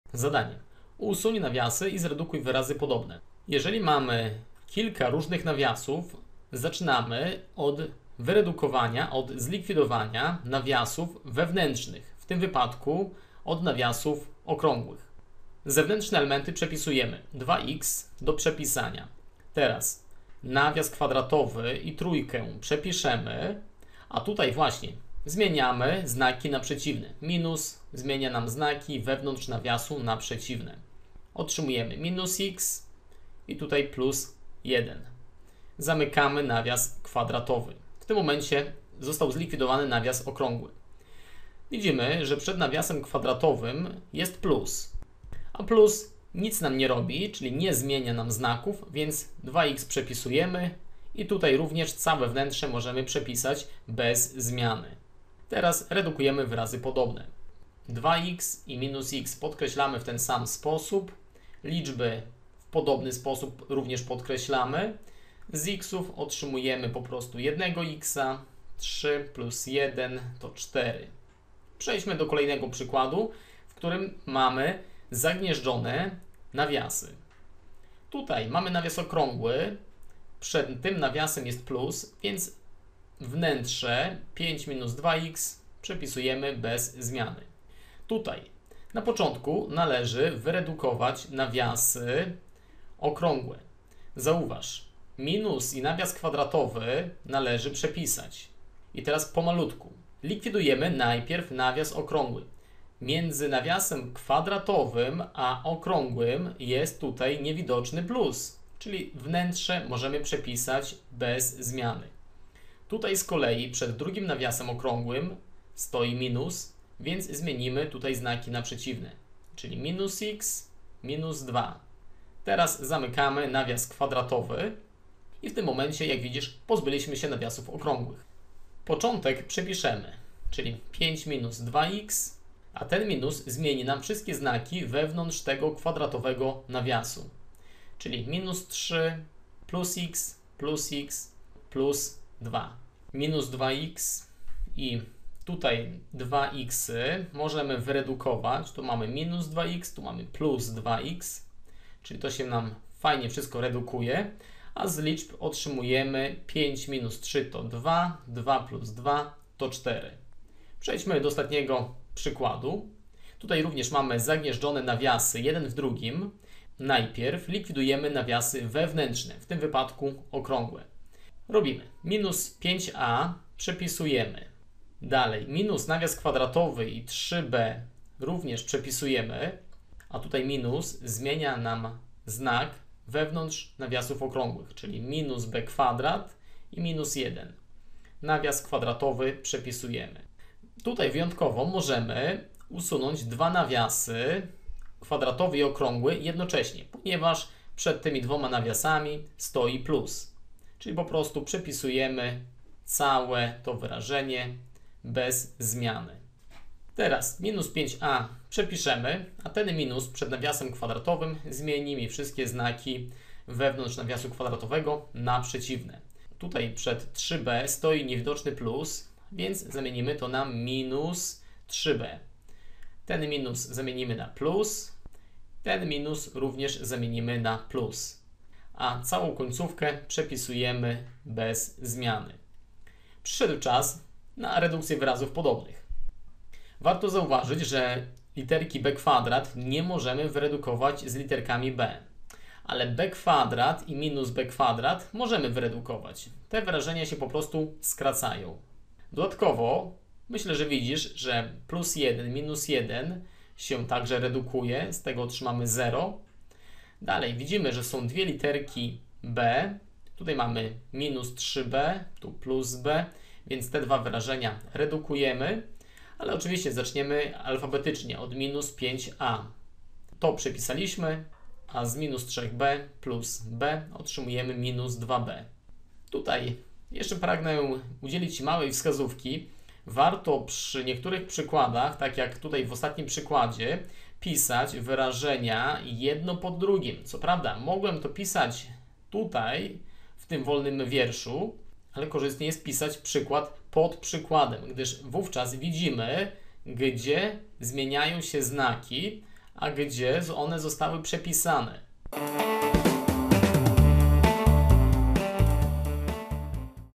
Redukcja wyrazów podobnych - Wyrażenia algebraiczne - Matfiz24.pl
W nagraniu dowiesz się wszystkiego co niezbędne odnośnie redukcji wyrazów podobnych.